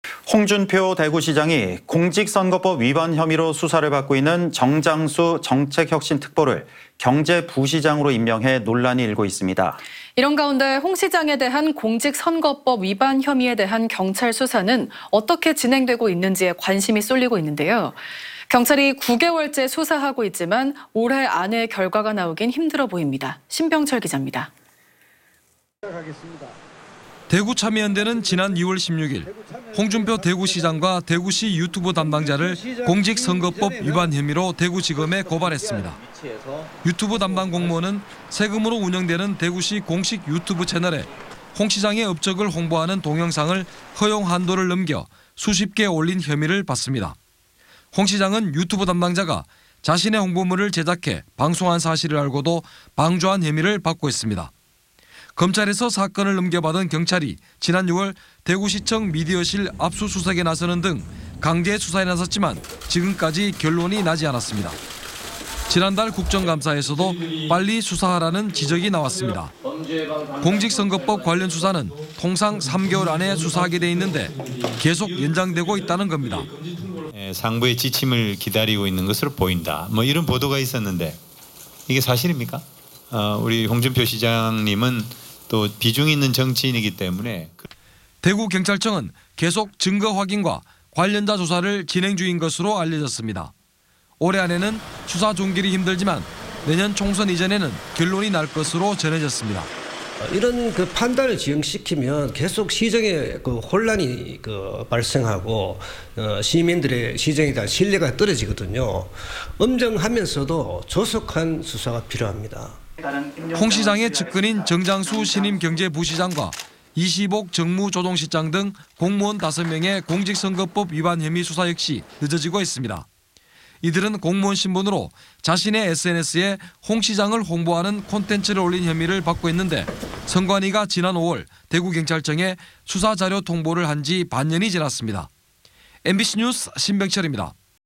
[R]홍준표 대구시장 공직선거법 위반 혐의 수사 해 넘기나? / 안동MBC
2023/11/23 08:30:00    작성자 : 보도팀장

◀ 앵 커 ▶
홍준표 대구시장이 공직선거법 위반 혐의로
수사를 받고 있는 정장수 정책혁신특보를
경제부시장으로 임명해 논란이 일고 있습니다.

이런 가운데 홍 시장에 대한 공직선거법 위반
혐의에 대한 경찰 수사는 어떻게 진행되고 있는지에 관심이 쏠리고 있는데요.

경찰이 9개월째 수사하고 있지만
올해 안에 결과가 나오긴 힘들어 보입니다.

보도에 심병철 기자입니다.
◀ END ▶

◀ 리포트 ▶
대구참여연대는 지난 2월 16일
홍준표 대구시장과 대구시 유튜브 담당자를
공직선거법 위반 혐의로
대구지검에 고발했습니다.

유튜브 담당 공무원은 세금으로 운영되는
대구시 공식 유튜브 채널에
홍 시장의 업적을 홍보하는 동영상을
허용 한도를 넘겨 수십 개 올린
혐의를 받습니다.

홍 시장은 유튜브 담당자가
자신의 홍보물을 제작해 방송한 사실을 알고도 방조한 혐의를 받고 있습니다.

검찰에서 사건을 넘겨받은 경찰이 지난 6월
대구시청 미디어실 압수수색에 나서는 등
강제 수사에 나섰지만 지금까지 결론이 나지 않았습니다.

지난 달 국정감사에서도 빨리 수사하라는
지적이 나왔습니다.

공직선거법 관련 수사는 통상 3개월 안에 수사하게 돼 있는데, 계속 연장되고 있다는 겁니다.

◀ INT ▶이해식/더불어민주당 국회의원
(10월 23일, 대구경찰청 국정감사 )
"상부의 지침을 기다리고 있는 것으로 보인다, 이런 보도가 있는데 사실입니까? 홍준표 시장님은 비중 있는 정치인이기 때문에..."

대구경찰청은 계속 증거 확인과 관련자 조사를 진행중인 것으로 알려졌습니다.

올해 안에는 수사 종결이 힘들지만 내년 총선 이전에는 결론이 날 것으로 전해졌습니다.

◀ INT ▶강금수/대구참여연대 사무처장
"이런 판단을 지연시키면 계속 시정의 혼란이 발생하고 시민들의 시정에 대한 신뢰가 떨어지거든요. 엄정하면서도 조속한 수사가 필요합니다."

홍 시장의 측근인 정장수 신임 경제부시장과
이시복 정무조정실장등 공무원 5명의
공직선거법 위반 혐의 수사 역시
늦어지고 있습니다.

이들은 공무원 신분으로 자신의 SNS에
홍 시장을 홍보하는 콘텐츠를 올린 혐의를
받고 있는데, 선관위가 지난 5월 대구경찰청에
수사자료 통보를 한 지 반년이 지났습니다.

mbc뉴스 심병철입니다.